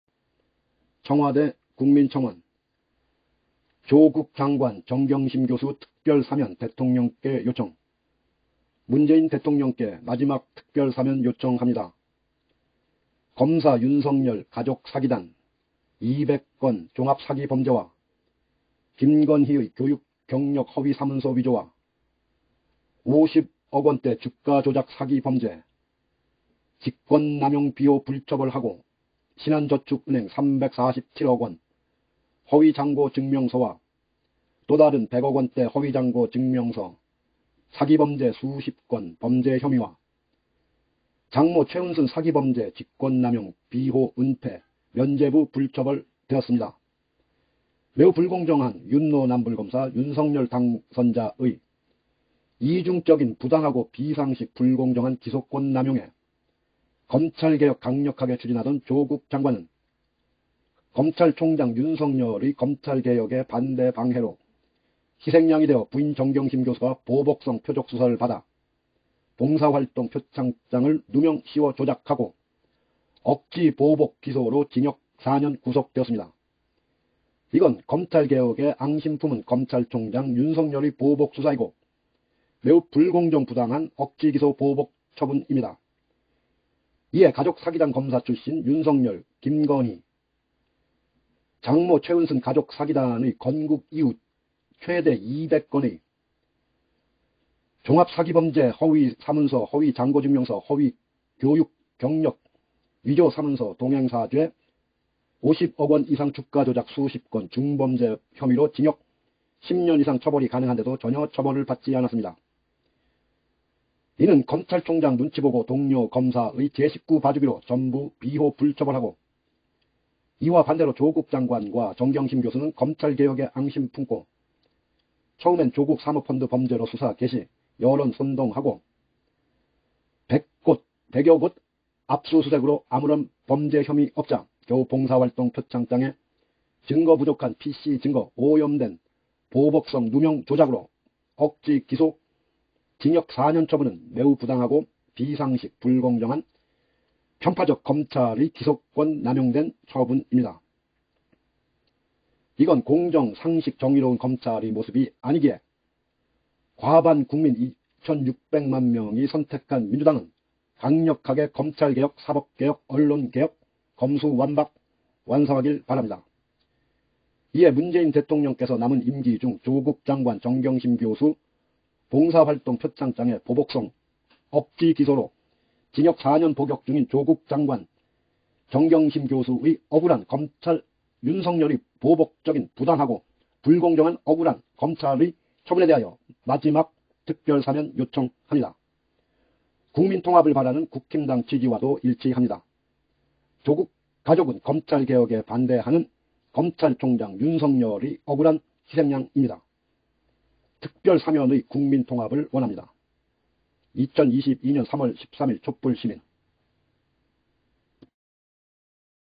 청와대 국민청원...조국장관 정경심교수 특별사면 대통령께 요청..
조국장관 정경심교수 특별사면 대통령께 요청

청원기간
22-03-13 ~ 22-04-12

문재인 대통령께 마지막 특별사면 요청합니다

검사 윤석열 가족사기단 200건 종합 사기 범죄와 김건희의 교육 경력 허위 사문서 위조와 50억원대 주가조작 사기범죄 직권남용 비호 불처벌하고 신안저축은행 347억원 허위 잔고증명서와 또 다른 100억원대 허위잔고 증명서 사기범죄 수십건 범죄 혐의와 장모 최은순 사기범죄 직권남용 비호 은폐 면죄부 불처벌 되었습니다.

매우 불공정한 윤로남불 검사 윤석열 당선자의 이중적인 부당하고 비상식 불공정한 기소권 남용에 검찰개혁 강력하게 추진하던 조국장관은 검찰총장 윤석열의 검찰개혁에 반대 방해로 희생양되여 부인 정경심교수가 보복성 표적수사를 받아 봉사활동 표창장을 누명씌워 조작하고 억지 보복기소 징역 4년 구속되었습니다. 이건 검찰개혁에 앙심품은 검찰총장 윤석열의 보복수사이고 매우 불공정 부당한 억지기소 보복 처분입니다.

이에 가족사기단 검사출신 윤석열 김건희 장모 최은순 가족사기단의 건국이후 최대 200건의 종합 사기범죄 허위사문서 허위잔고증명서 허위교육경력 위조사문서 동행사죄 50억원이상 주가조작 수십건 중범죄 혐의로 징역 10년이상 처벌이 가능한데도 전혀 처벌을 받지 않았습니다.

이는 검찰총장 눈치보고 동료 검사의 제식구봐주기로 전부 비호 불처벌하고 이와 반대로 조국장관과 정경심 교수는 검찰개혁에 앙심품고 처음엔 조국 사모 펀드 범죄로 수사개시 여론선동하고 100곳 압수수색으로 아무런 범죄 혐의없자 겨우 봉사활동 표창장에 증거 부족한 PC증거 오염된 보복성 누명 조작으로 억지기소 징역 4년 처분은 매우 부당하고 비상식 불공정한 편파적 검찰의 기소권 남용된 처분입니다

이건 공정 상식 정의로운 검찰의 모습이 아니기에 과반 국민 1천6백만명이 선택한 민주당은 강력하게 검찰개혁 사법개혁 언론개혁 검수완박 완성하길 바랍니다.

이에 문재인 대통령께서 남은 임기중 조국장관 정경심교수 봉사활동 표창장에 보복성 억지기소로 징역 4년 복역중인 조국장관 정경심 교수의 억울한 검찰 윤석열의 보복적인 부당하고 불공정한 억울한 검찰의 처분에 대하여 마지막 특별사면 요청합니다.

국민통합을 바라는 국힘당 취지와도 일치합니다
조국가족은 검찰개혁에 반대하는 검찰총장 윤석열의 억울한 희생양 입니다. 특별사면의 국민통합 원합니다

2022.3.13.

촛불 시민